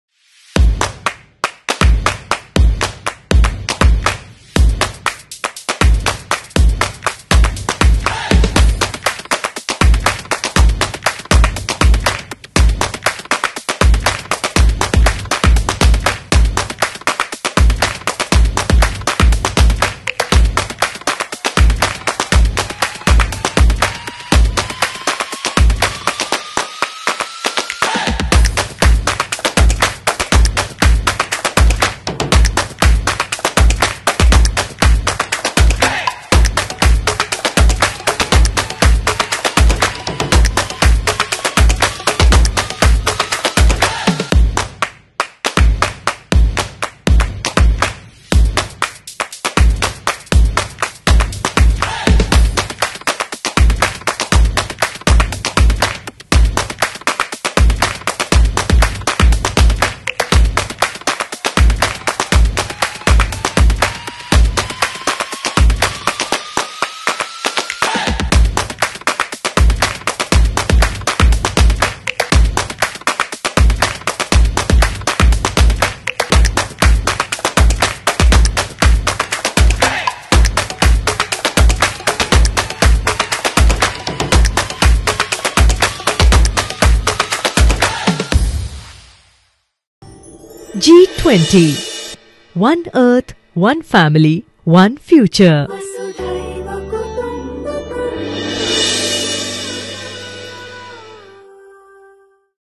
What is Culture?
From unique beliefs and traditions to language and art to music and cuisine, culture is a rich tapestry that shapes our identities, drives economic growth, fosters innovation, and creates jobs.

Come with us as the G20 Culture Working Group under G20India meets to discuss ways to integrate culture, a vital cornerstone of our societies, in global policies.